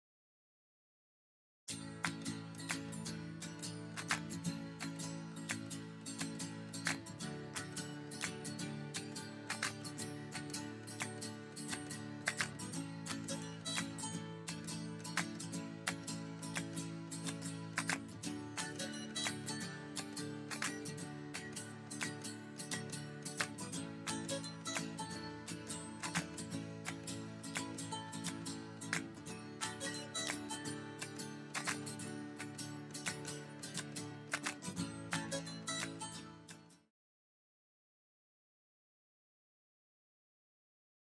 SIMPLE SALMON SALAD - Student Recipe
Student recipes: A super simple Salmon Salad that makes for a healthy student recipe.

MUSIC
TimBeek.com